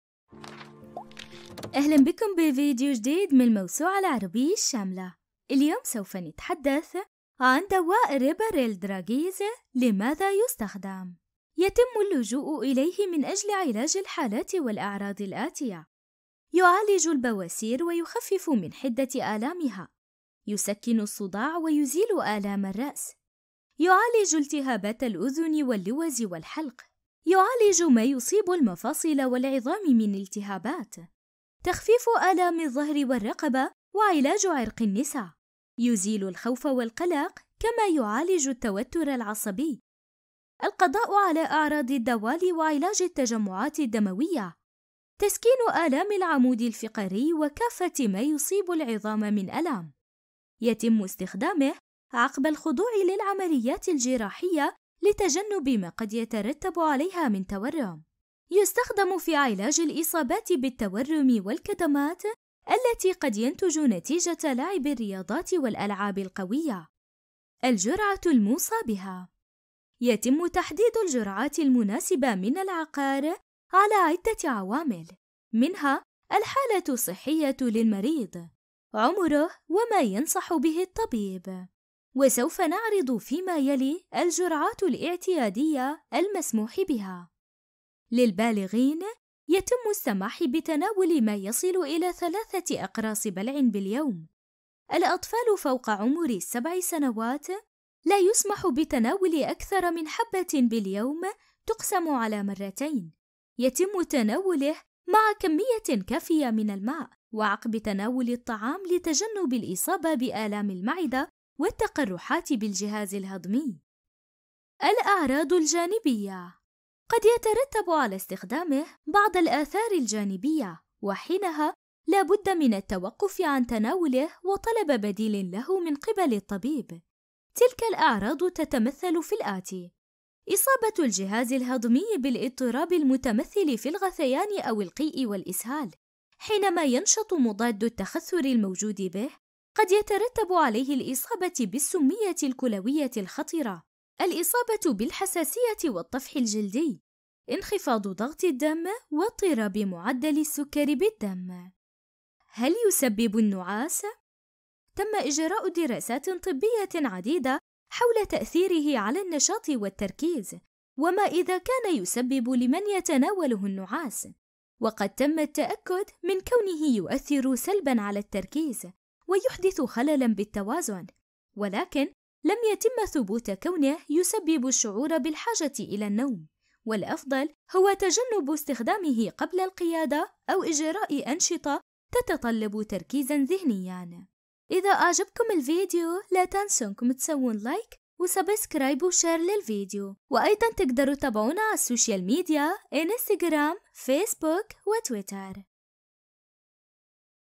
دواعي استعمال ريباريل "reparil dragees" والجرعة الصحيحة
يدور مقالنا التالي عبر موسوعة حول دواء reparil dragees لماذا يستخدم وهو من الأدوية التي يتم اللجوء إليها كثيراً من أجل تخفيف الأوجاع وتسكين الآلام وقد أثبت مدى ما له من فعالية في ذلك الشأن، كما يعد عقاراً فعالاً في التخلص مما يصيب الجسم من التهابات، حيث تعد المادة الفعالة هي الأسين والتي تتواجد به بتركيز أربعين ملجم.
للمزيد من المعلومات عن دواعي استعمال ريباريل "reparil dragees" والجرعة الصحيحة  يمكنكنك متابعة
أو
reparil dragees اعراض جانبية

للبحث عن أي موضوع يهمك يمكنك البحث في